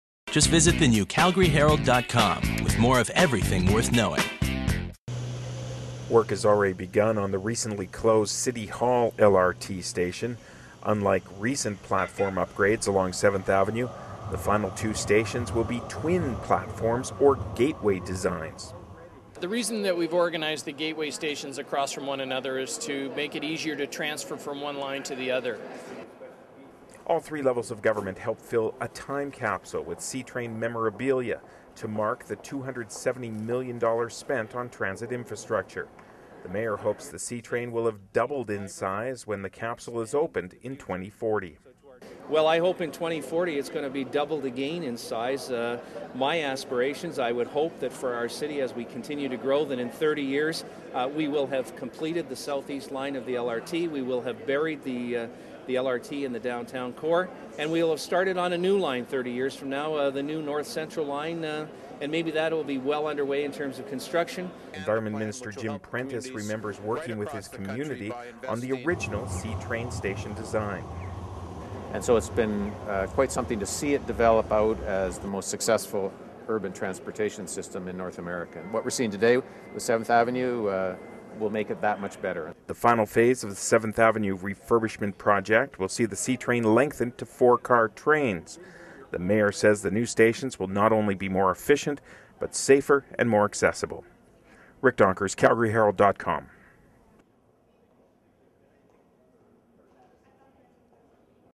City unveils LRT gateway stations
C-Train riders will see a new platform design when the city hall station re-opens next year. Both ends of the 7th Avenue transit corridor will be serviced by twinned 'gateway&quot; stations with platforms on each side of the street. Rick Donkers reports.